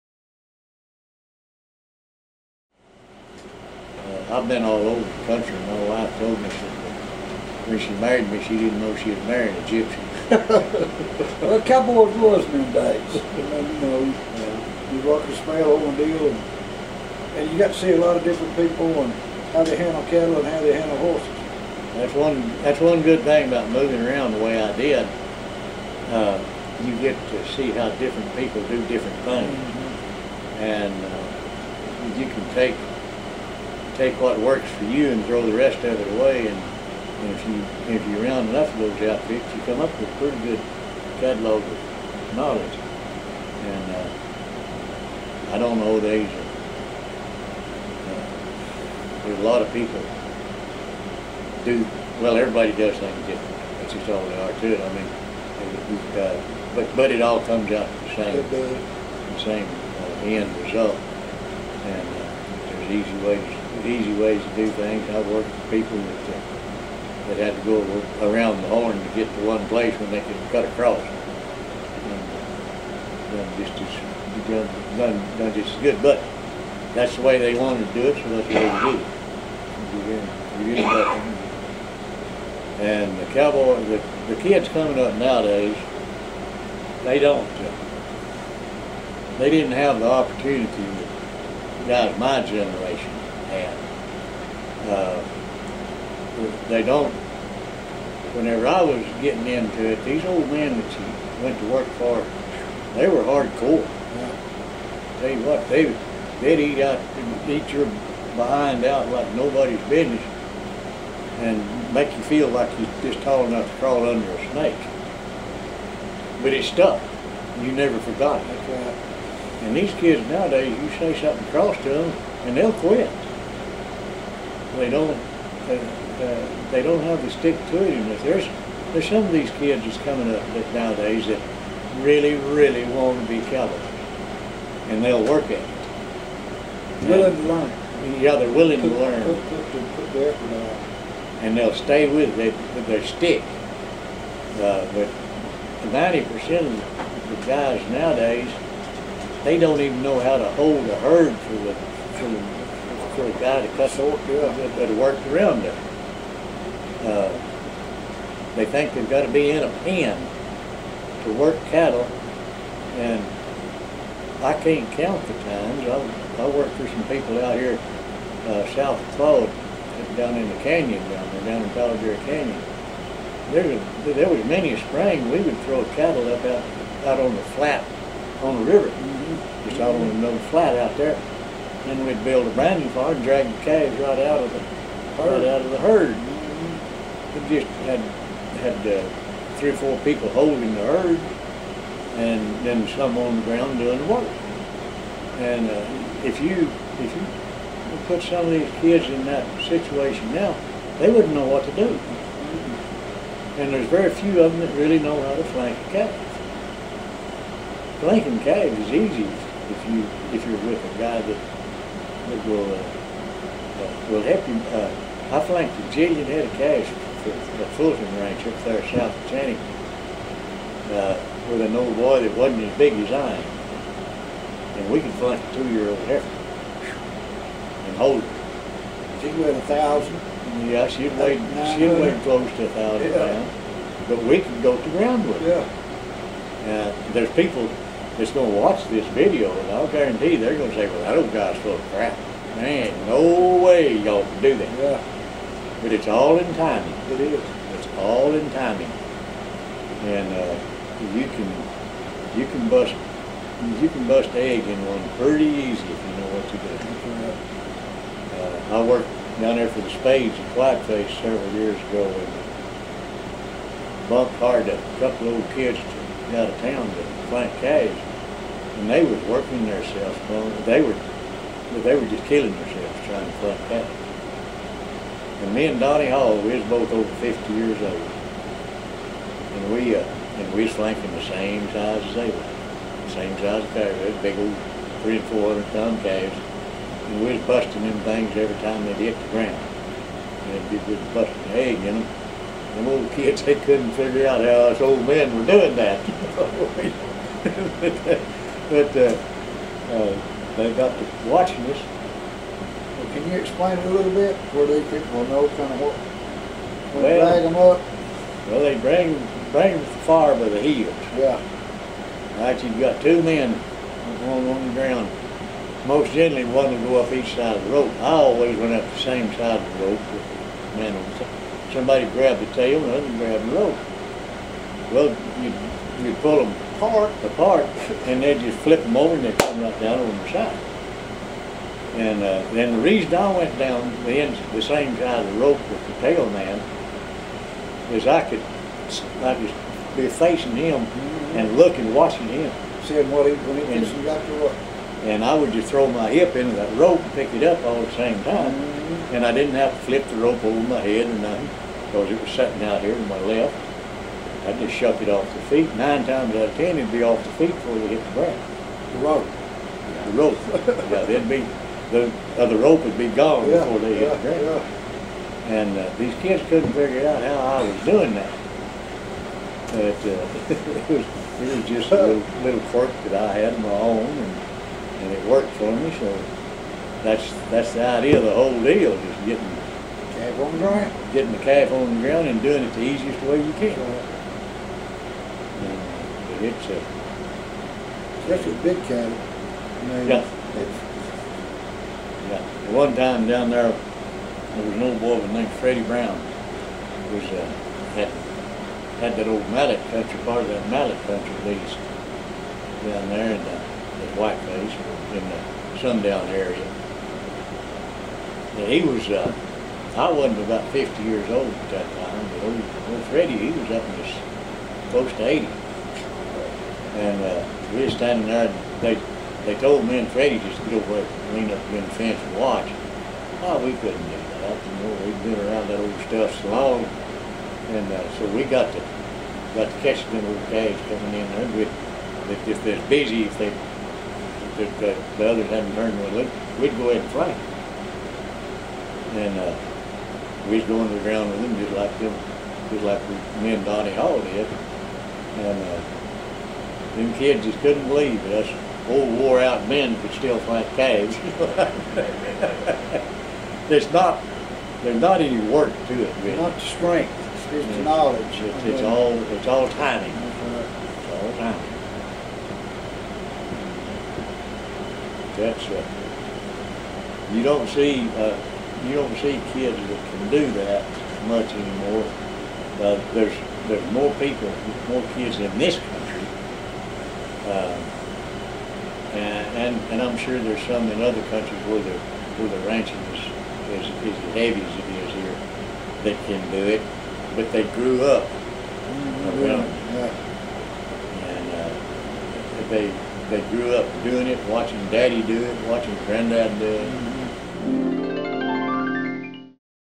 Punch Skelton: Today's Millennial Cowboys 2/9
Buster Bruce joins me in rounding up some older West Texas cowboys in order to preserve their stories and memories. Punch has spent a lifetime working on cattle ranches throughout the Great Plains in the United States. He reflects back on his own experiences and comments on a concern that young cowboys don't have what it takes in order to make a competent ranch hand.

This video is a documentary effort of the CaptiveImage.us Heritage Preservation Project